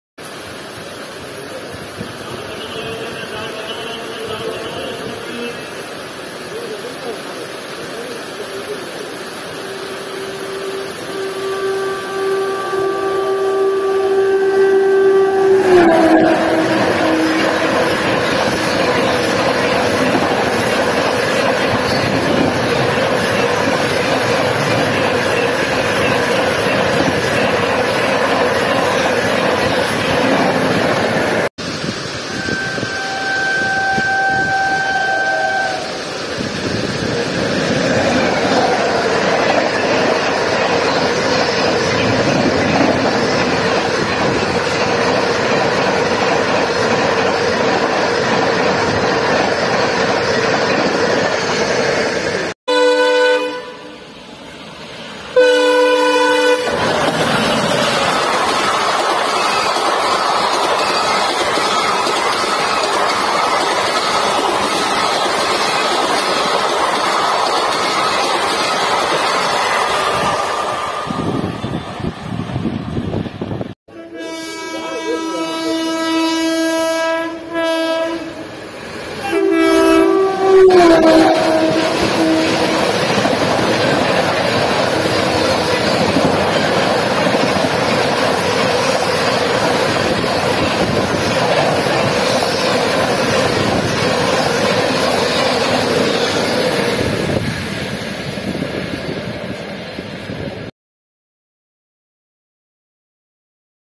TRAIN IN HEAVY RAIN !! High Speed Trains 130 kmph One amazing Day !
Trains Features in this Video

00:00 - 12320 Agra Cantt Kolkata SF Express
  12320 आग्रा कँट्ट कोलकाता एक्स्प्रेस

00:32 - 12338 Bolpur Shantiniketan Howrah Jn Shantiniketan Express
  12338 बोलपुर शांतीनिकेतन हावडा जं शांतीनिकेतन एक्स्प्रेस

00:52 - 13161 Kolkata Balurghat Tebhaga Express
13161 कोलकाता बालुरघाट तेभागा एक्सप्रेस

01:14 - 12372 Bikaner Jn Howrah Jn SF Express
12372 बिकानेर जं हावडा जं एक्सप्रेस

01:41 - Thanks for Watching Subscribe